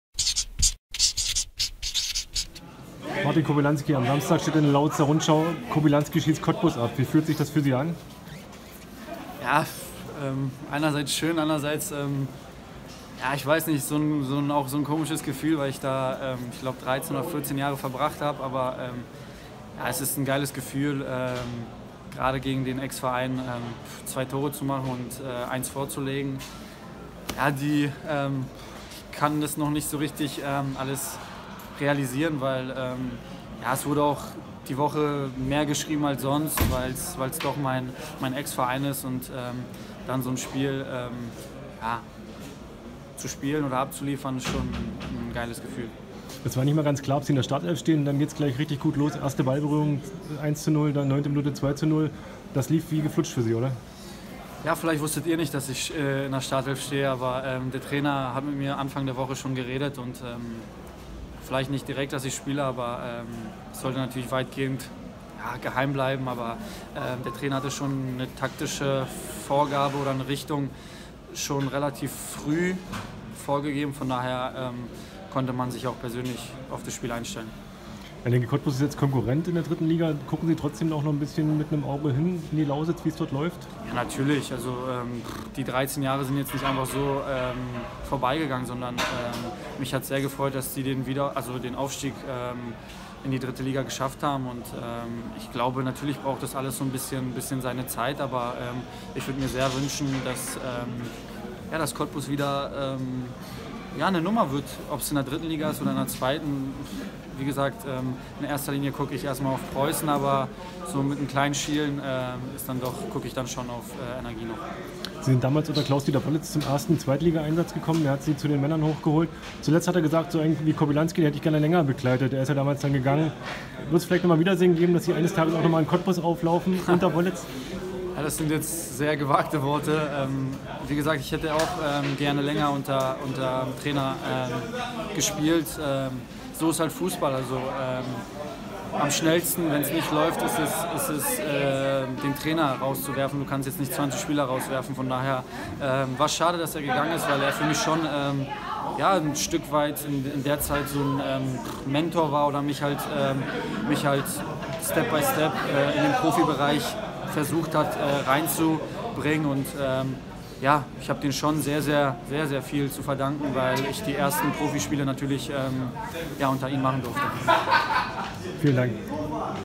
Martin Kobylanski nach dem Doppelpack gegen Energie Cottbus
Ausgerechnet das Cottbuser Eigengewächs Martin Kobylanski hat beim 3:0 des SC Preußen Münster gegen Energie Cottbus zwei Tore erzielt und das dritte vorbereitet. Im RUNDSCHAU-Interview spricht er über dieses besondere Spiel, die Vergangenheit in Cottbus und seine Zukunft.